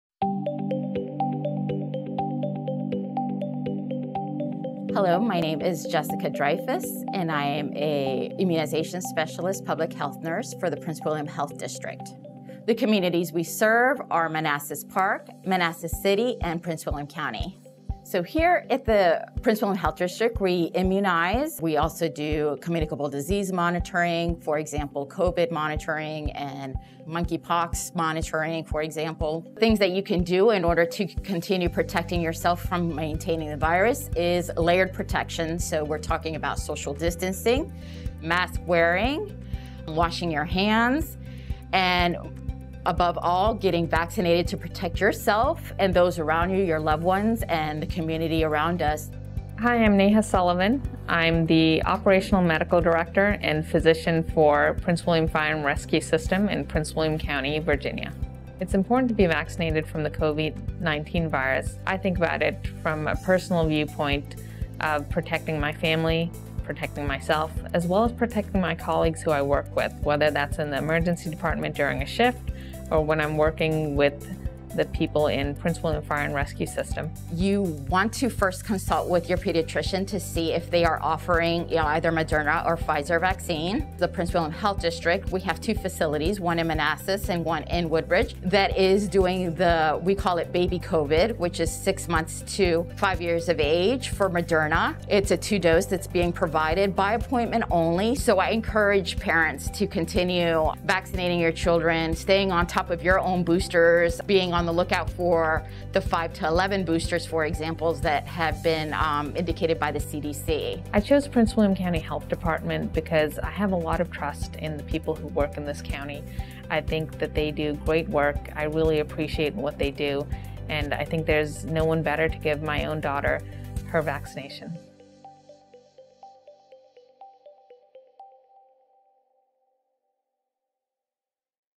Prince William Health District – COVID-19 Vaccinations for Children Under 5
Prince William Health District Now Giving Vaccinations to Young Children

The Prince William Health District is now offering COVID-19 vaccinations, by appointment, for children from six months to five years old at its Manassas and Woodbridge offices.

“We waited a very long time for this vaccination,” said the Operating Medical Director for the Prince William Fire and Rescue System, Dr. Neha Sullivan. “We’re relieved that it’s finally here. We’re looking forward to spending more time with friends and family. We’ve taken a lot of precautions over the last three years of the pandemic to protect our loved ones.”

Getting children vaccinated can free families who want to finally visit loved ones and friends who might be frail, elderly relatives with preexisting conditions or those who are immunocompromised.

“We want to make sure that when we go visit them, we’re not putting them at risk,” said Sullivan who recently had her young daughter vaccinated.

The Prince William Health District, which serves Prince William County and the cities of Manassas and Manassas Park, has seen a good response since it began offering vaccines for younger children in June.

“We have seen an increase in … requests for vaccines in this age group which is a good thing because it means that we are protecting our community the best way we can,” said Prince William County Health District Immunization Specialist Jessica Dreyfuss. “With that said, we are also relying on a lot of our partners in our community — pediatricians — to provide vaccines to this age group.”

While many young children seem to weather COVID-19 with few ill effects, it is still critical to consider vaccines.

“It’s important for young children under the age of five to be vaccinated for a few reasons. It’s both the immediate effects of COVID which can be a serious illness — hospitalization, emergency visits, time away from school and other activities they love as well as the long-term effects we’re seeing in children.  Long COVID can include among other symptoms difficulty concentrating, chronic fatigue and long covid itself might lead to greater risk of inflammatory conditions of the heart and blood clots,” Sullivan said.

Parents who have concerns about getting their children vaccinated with either the Moderna or Pfizer-BioNTech vaccines should talk to their pediatricians and ask questions for clarification and education so they can be comfortable in getting their children vaccinated. The Centers for Disease Control and Prevention, or CDC, also offers information about the vaccine, trial results and statistics.

“When I think about this on a personal level, I want things that are protecting my family, protecting myself as well as protecting the colleagues I work with, whether that’s in the emergency department during a shift or the vulnerable patients I see,” Sullivan said.

To guard against getting COVID-19 people should continue to practice COVID-19 safety protocols.
“The community has been doing very well protecting themselves from the COVID-19 virus with layered prevention, so handwashing, social distancing, mask wearing and, above all, getting vaccinated to protect yourself, your loved ones and the community around us,” Dreyfuss said.

To make an appointment for a vaccination call for the Health District’s Manassas Office at 9301 Lee Avenue in Manassas or for the Woodbridge office at 4001 Prince William Parkway, Suite 101.

for more information about vaccinating young children.